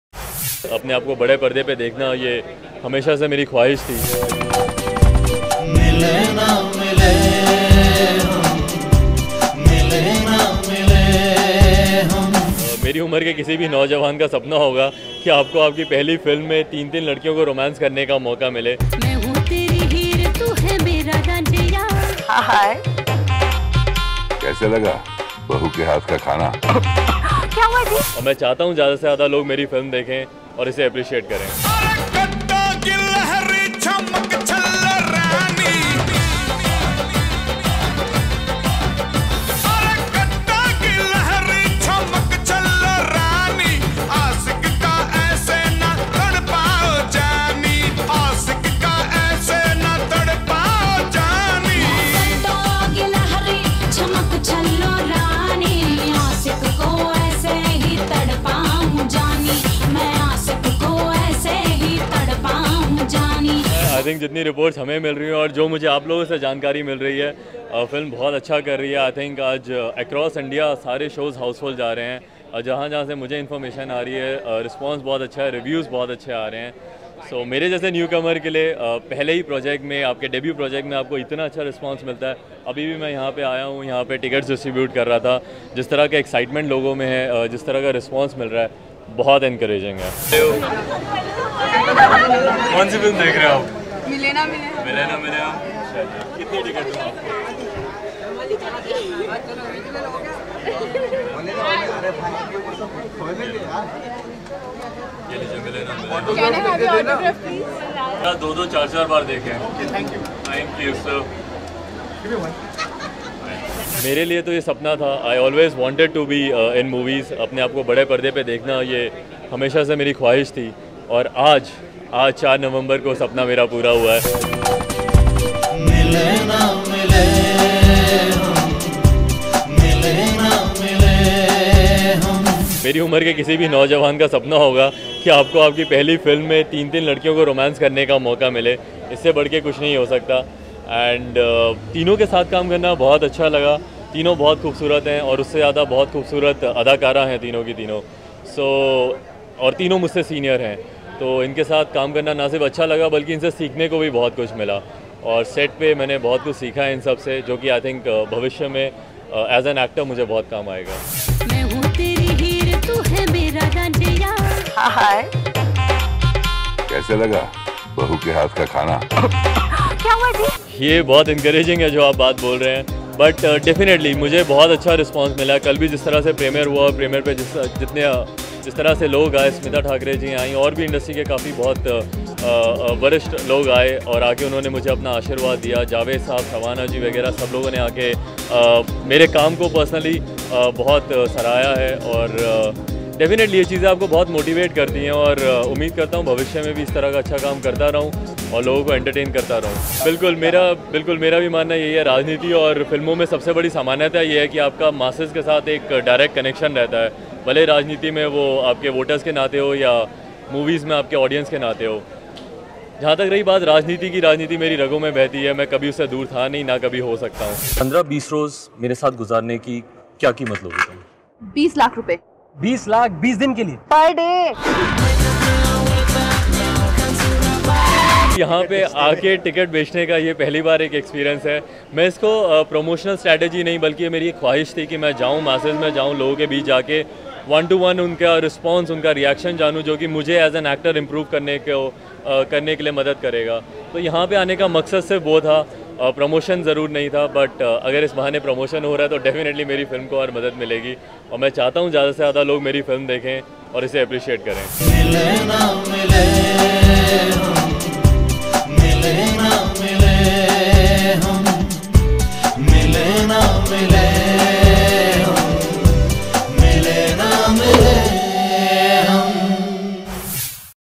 Chirag Paswan Sells Tickets Of 'Miley Naa Miley Hum'
Actor Chirag Paswan was seen selling the tickets of his debut film 'Miley Naa Miley Hum'. He spoke about people's response and more.